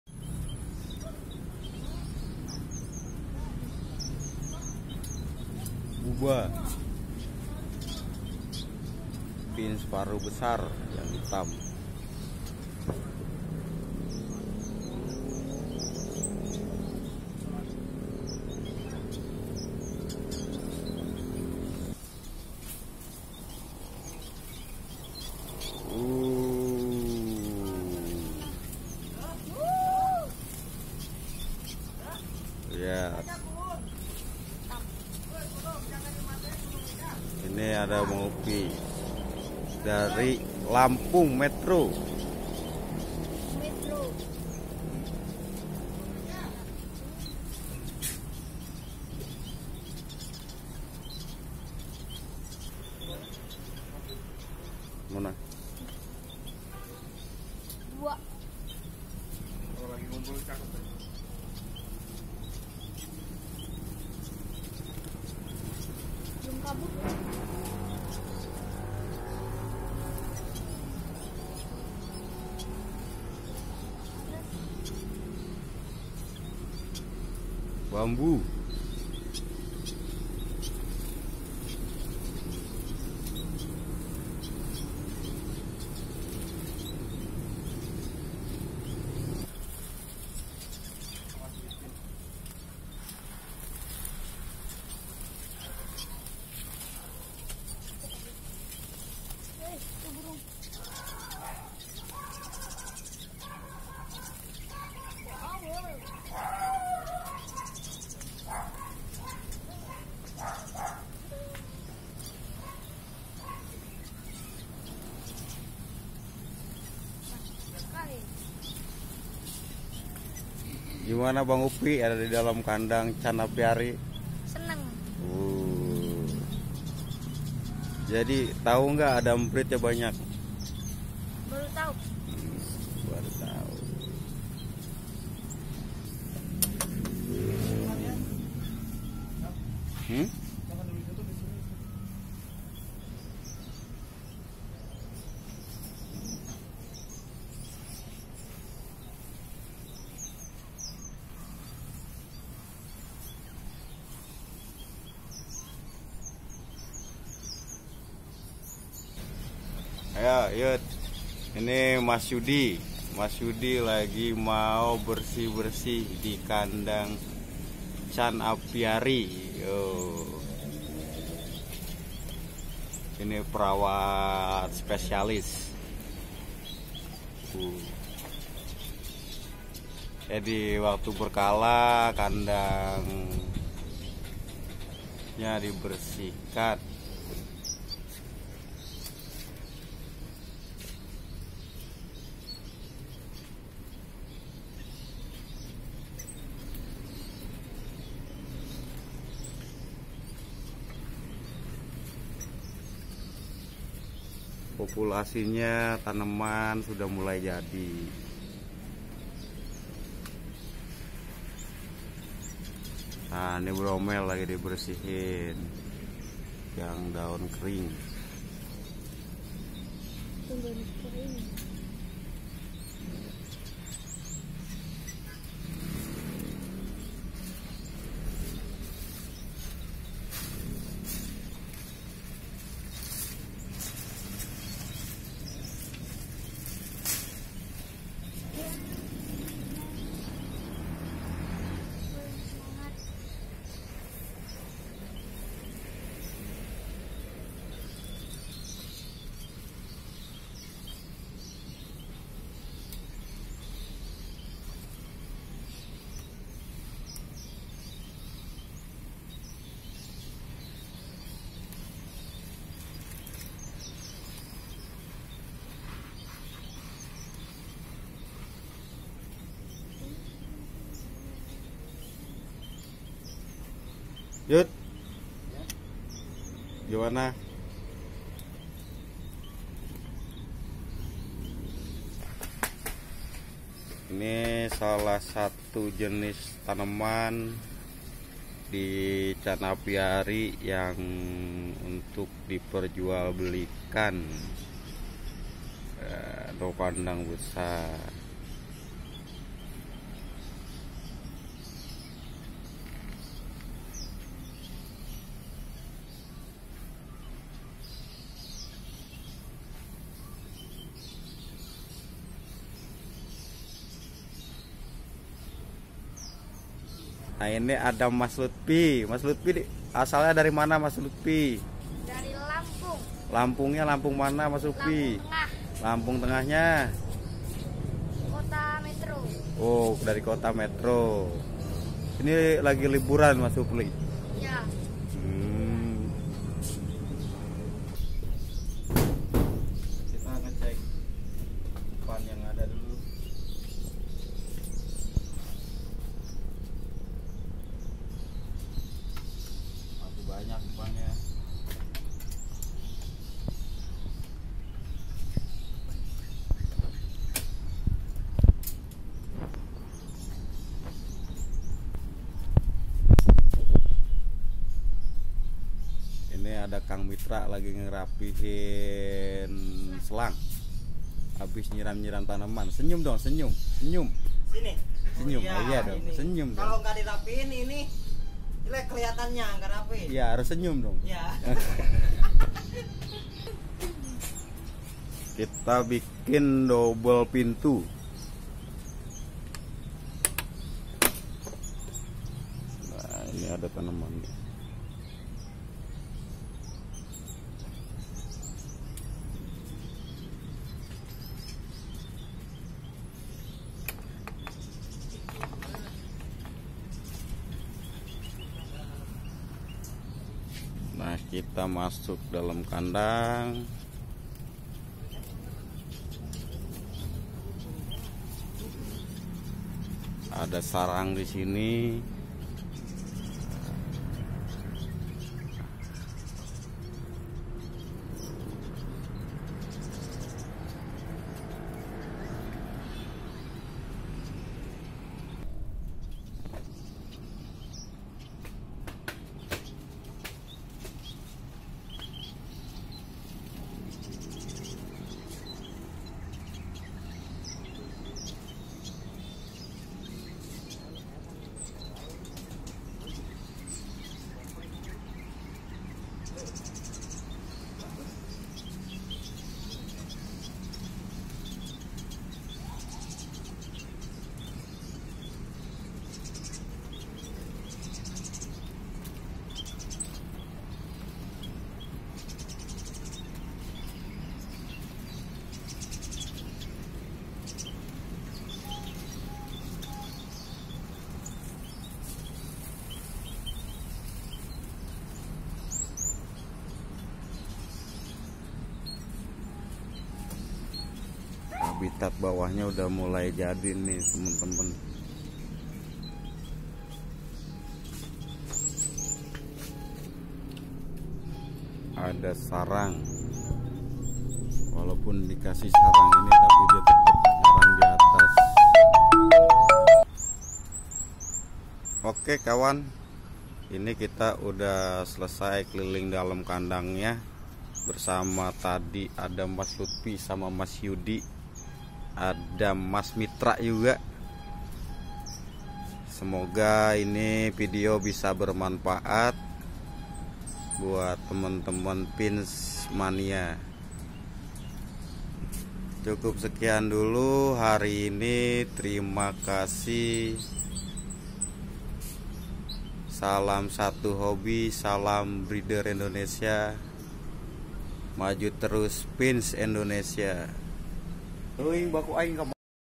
chan aviary# penangkaran finc/emprit/pipit
jenis finc lokalan indonesia dgn luas kandang dgn ukuran P 5m L 3,5m T 2,70m dgn isi kapasitas burung 15 pasang